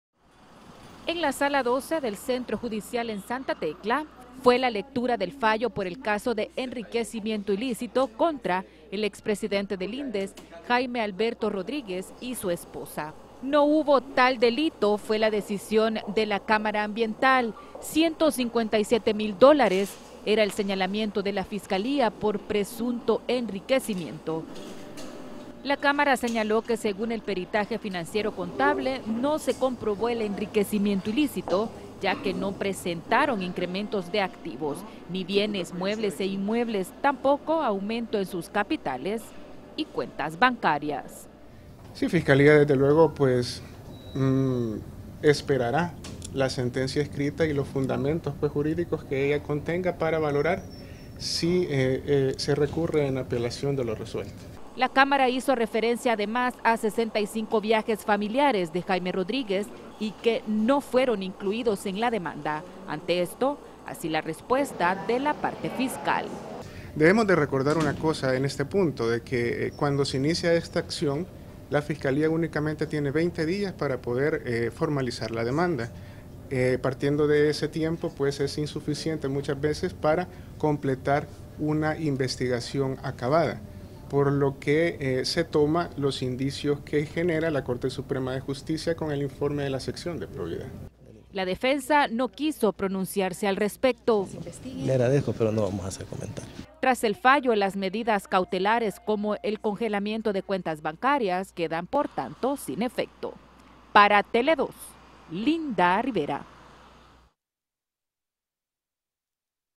Resolución, expresidente del INDES
El expresidente del INDES, Jaime Rodríguez y su esposa, no tendrán
que retribuir fondos al Estado pues fue desestimado el delito de
Enriquecimiento Ilícito